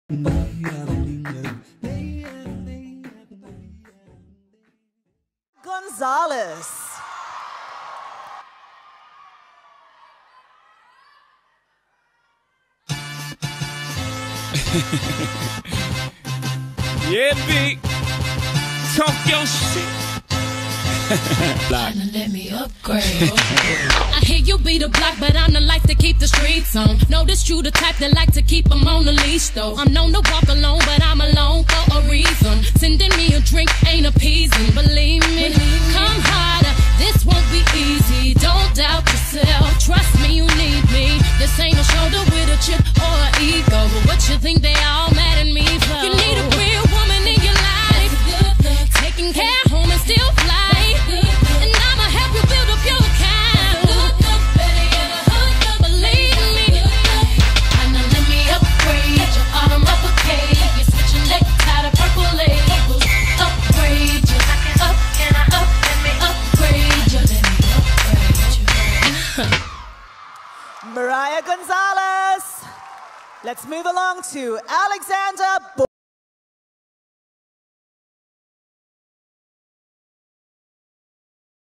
2018 Australian Dance Festival: Mariah Gonzales
Dance Around the World Competition performed at the 2018 Australian Dance Festival - A Celebration of Australia's Best in Dance at the Sydney Olympic Park Sports Centre. Get set for 2019! Dates are already confirmed as the 19th - 22nd September 2019.